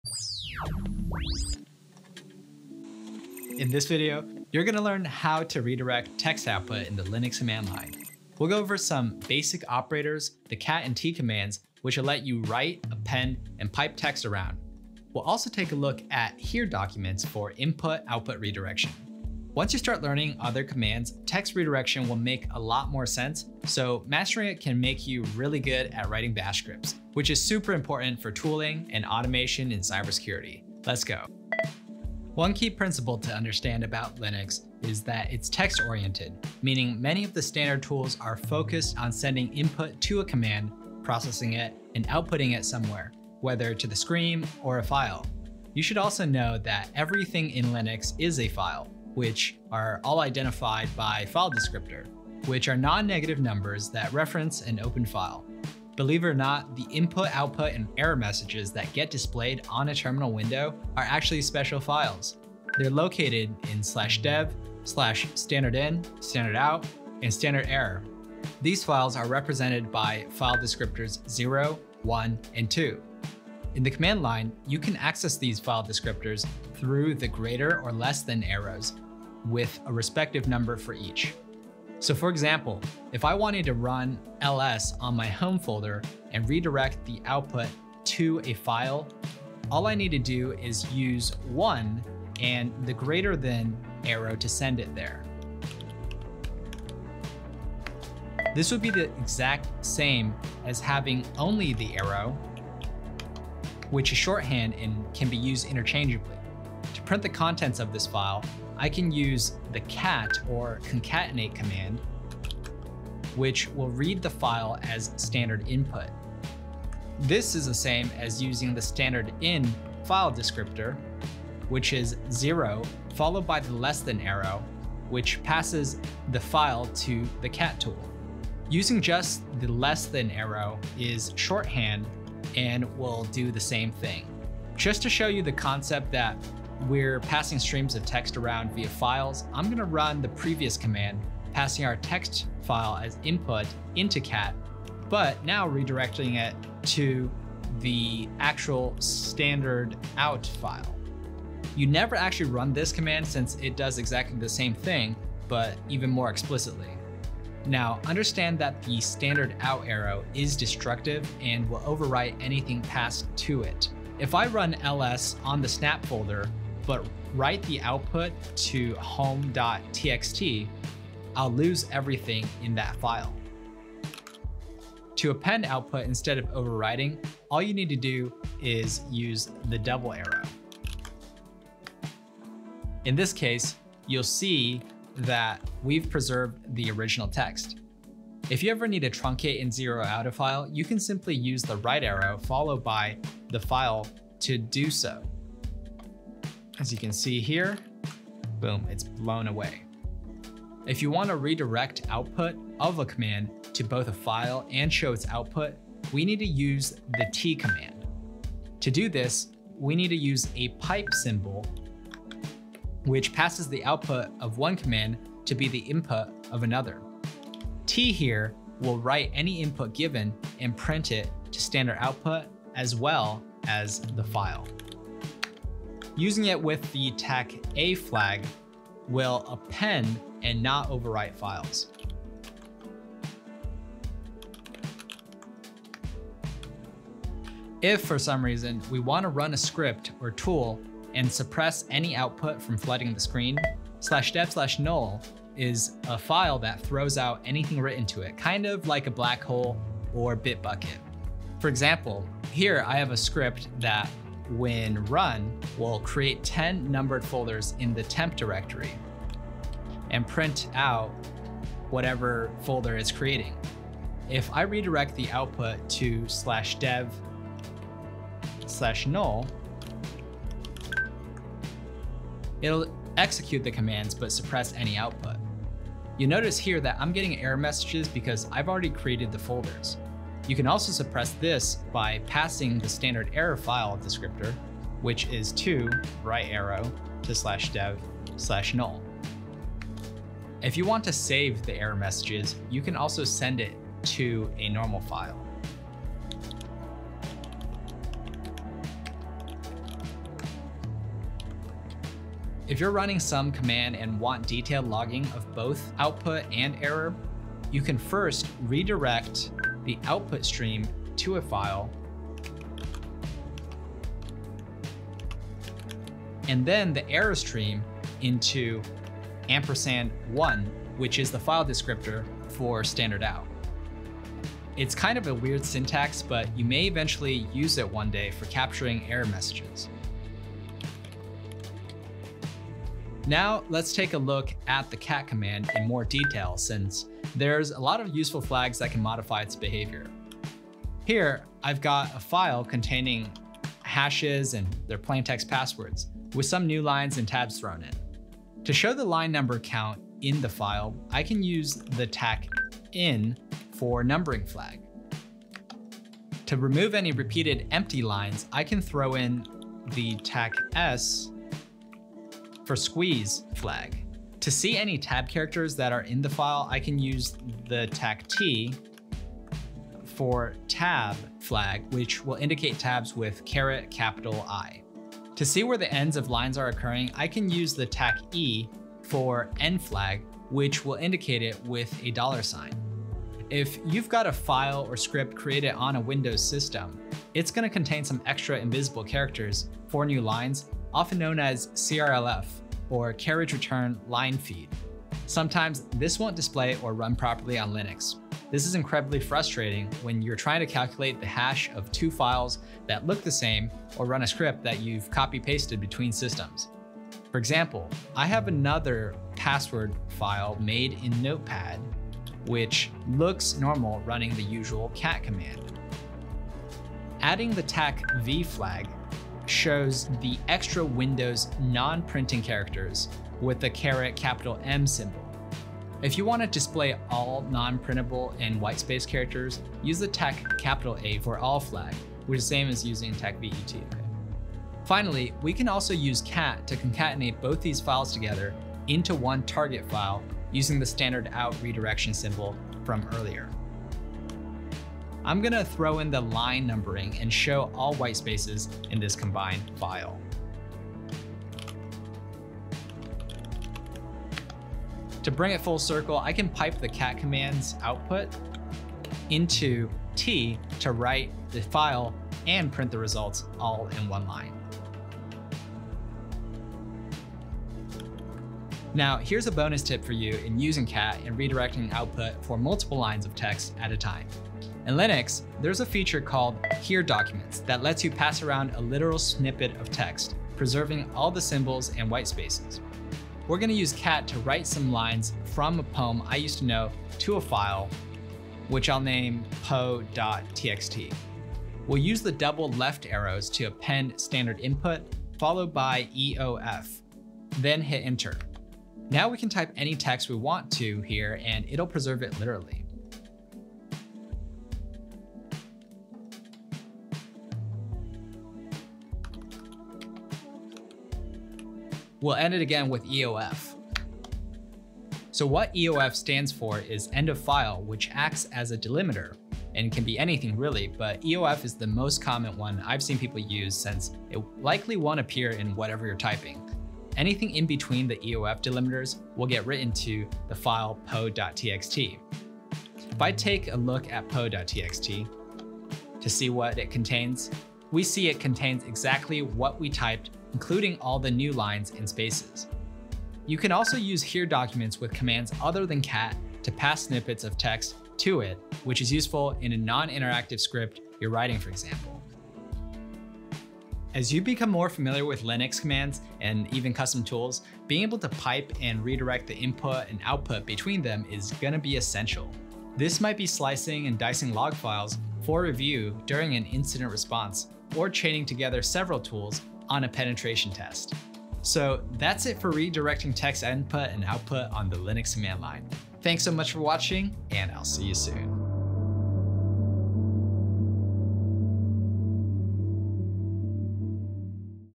How to Redirect Text Input/Output in Linux (cat, tee)
Redirecting text and data in the Linux command-line is an essential part of system administration. In this video you'll learn how to use cat, tee, and the Bash redirection symbols. This will let you pass data between the standard out, standard in, and standard error file descriptors.

Scripting is an essential part of cybersecurity. Almost every command line tool has some kind of text output, including error messages. If you want to capture these messages to a log file or pipe them to other commands, redirection is the answer.

In Linux, every is a file, from links, sockets, to devices. What's printed on the terminal (standard out) is a special device file located at /dev/stdout. Error messages (standard error) are stored in /dev/stderr. Terminal input (standard error) are in /dev/stdin.

To better access these buffers, bash conveniently provides several shortcuts that references them.
- The greater than arrow writes to standard out.
- The greater than arrow with a 2 before it writes to standard error
- The less than arrow reads from standard in.

When you display the contents of a file using the cat command, Bash actually passes the file's contents as input into the tool. Cat will then write the contents to the standard out file, which displays on the screen.

When you pipe a command's output to the tee command, it will write it to both standard out and a file of your choice.

In essence, anything that can be read or written from the terminal screen can be passed to a file. And vice versa.

00:00 Introduction
00:33 Key Principles for Bash Redirection
00:49 Linux File Descriptors
01:35 Redirecting Standard Output to a File
01:50 Displaying a File with Cat
02:00 Redirecting Data to Standard Input
02:55 Appending Output to a File
03:08 Truncating/Zeroing a File
03:27 Tee Command (Write to Standard Out and a File)
04:20 How to Use /dev/null
05:10 Redirecting Standard Error to Standard Out
05:40 Cat: Display Special Characters
07:57 How to Redirect Multiple Lines with Here Documents

We’re open to ideas and suggestions for more commands.